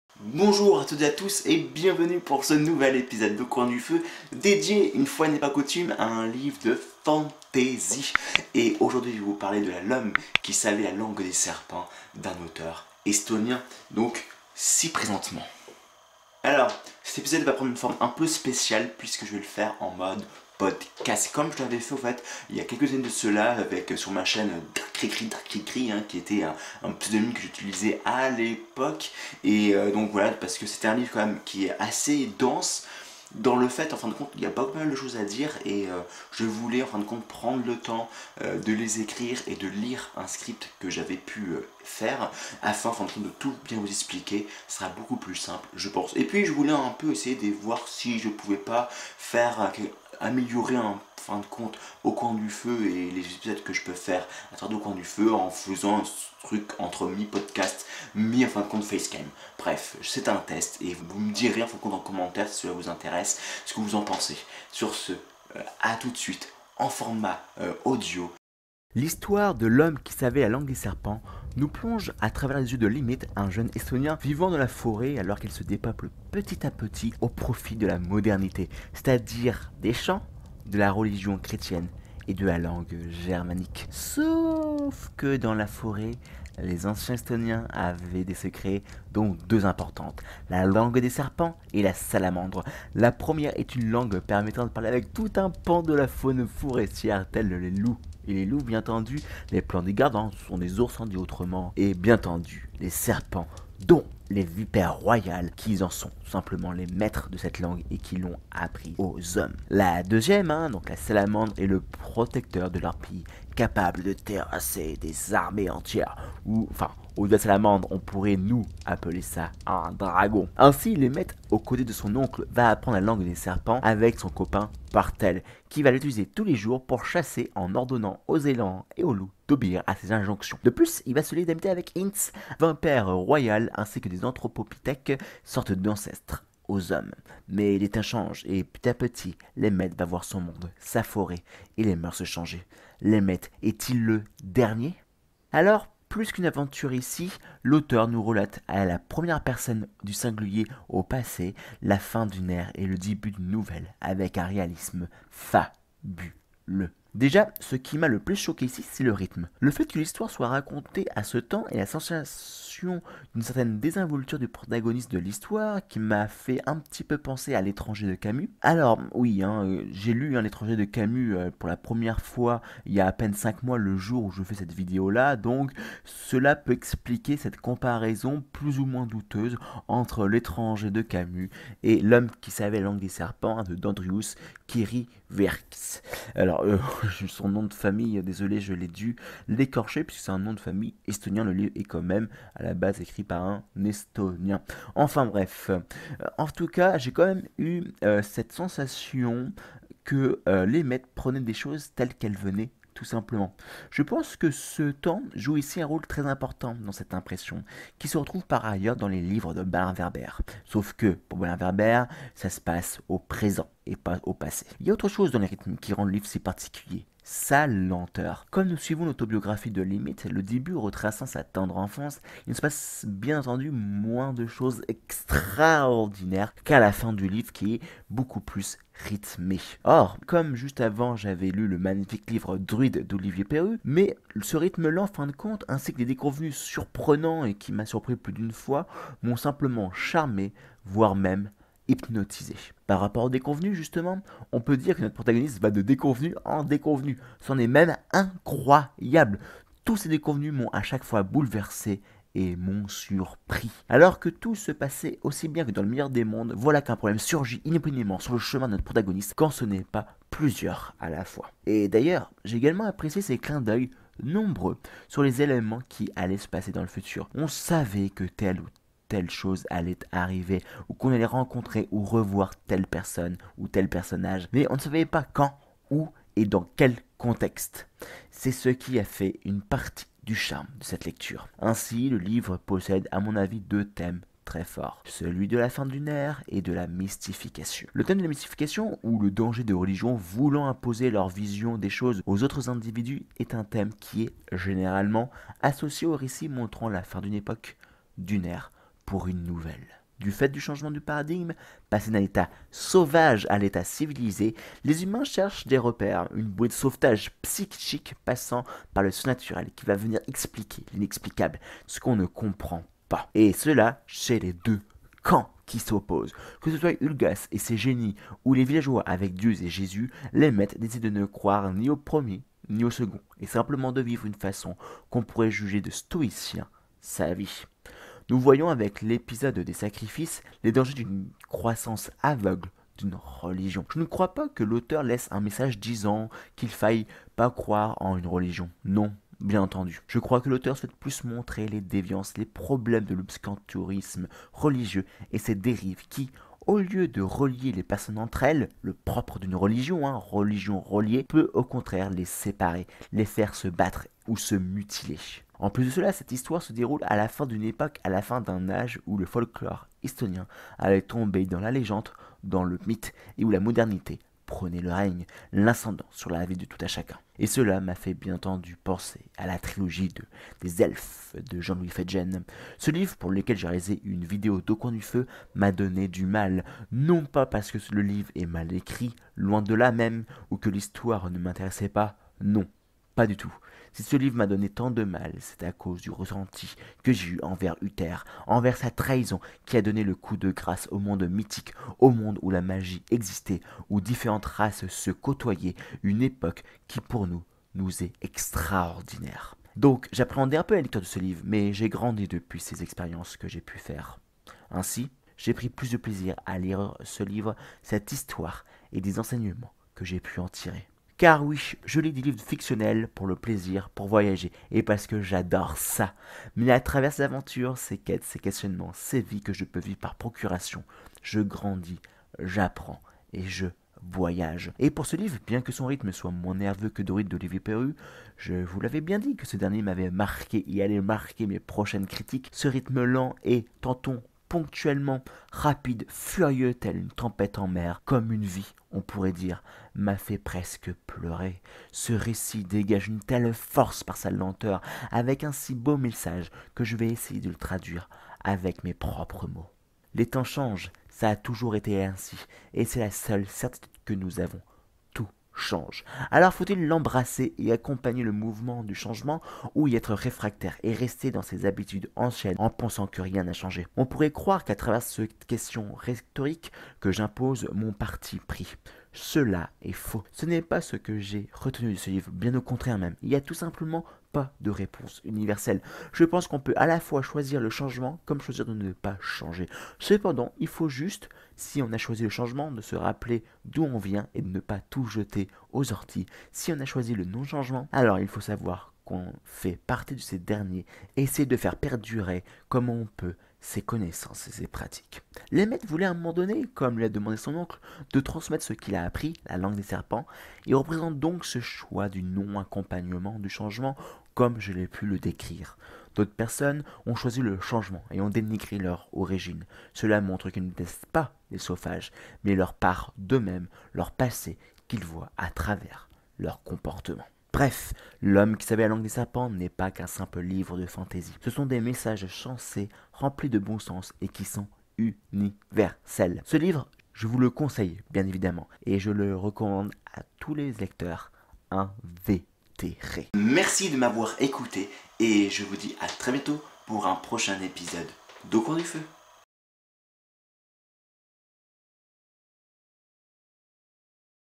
Au Coin du Feu #56 - L'homme qui savait la langue des serpents d'Andrus Kivirähk
L’homme qui savait la langue des serpents est une de ses histoires qu’on ne rencontre que trop rarement ! Un rythme lent mais hypnotisant qui s’accélère subitement sans qu’on s’en rende compte. Et s’il n’y avait que ça ! Les thèmes abordés tel la fin d’une époque et la mystification rend cette histoire juste belle. Pour tout vous dire j’ai bien failli pleurer à la fin de ce livre !

0:00 – Introduction
1:08 – Résumé de l’histoire
2:25 – Le temps et le rythme du livre
4:05 – Les déconvenues
4:45 – Les thèmes forts du livre
6:51 – Les romans comme moyen d’apprendre sur soi et sur le monde
7:30 – Mon interprétation du message du livre
8:53 – Conclusion